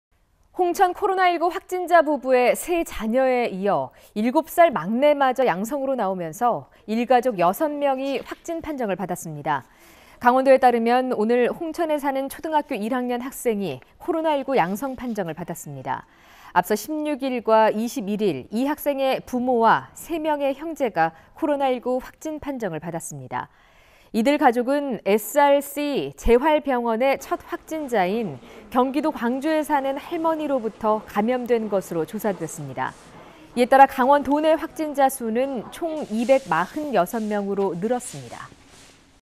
2020. 10. 22 [원주MBC] 홍천 일가족 6명 모두 확진..강원 누적 246명
홍천 코로나 19 확진자 부부의 세자녀에 이어 7살 막내마저 양성으로 나오면서 일가족 6명이 확진 판정을 받았습니다.

 강원도에 따르면, 오늘(22일) 홍천에 사는
초등학교 1학년 학생이 코로나19 양성 판정을 받았습니다.

 앞서 16일과 21일, 이 학생의 부모와 세명의 형제가 코로나19 확진 판정을 받았습니다.

 이들 가족은 SRC재활병원의 첫 확진자인
경기도 광주에 사는 할머니로부터
감염된 것으로 조사됐습니다.

 이에 따라 강원도 내 확진자 수는
총 246명으로 늘었습니다.